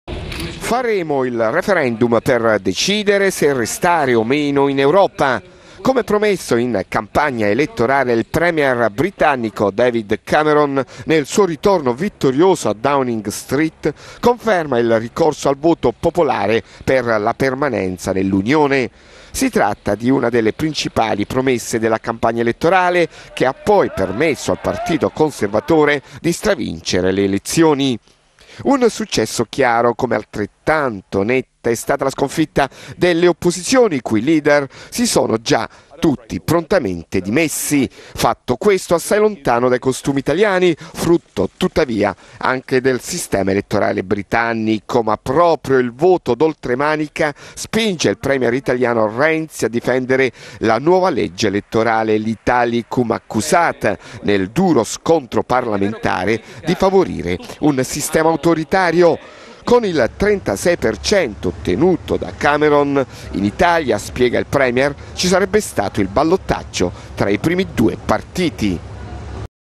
Dopo vittoria del partito conservatore Gran Bretagna valuta l'uscita dall'Unione Europea
La Gran Bretagna sembra un po' più lontana dall'Europa dopo la vittoria del partito conservatore che ha puntato la sua campagna sulla promessa di un referendum per uscire dall'unione. Intanto il premier Renzi per rispondere ad alcune critiche ipotizza quello che sarebbe accaduto in Gran Bretagna se si fosse votato con la nostra nuova legge elettorale, il tanto discusso "italicum". Sentiamo Augusto Cantelmi